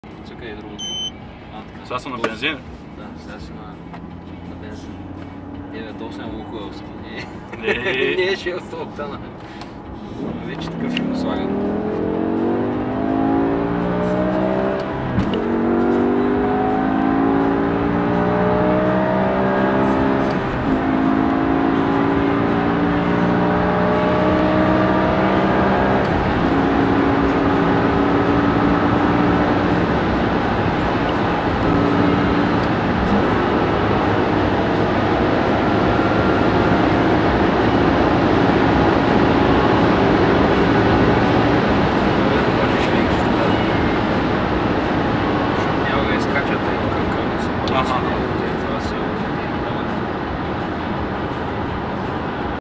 BMW E60 M5 50 - 250 km/h
Public road, low traffic, three people inside... acceleration can be much better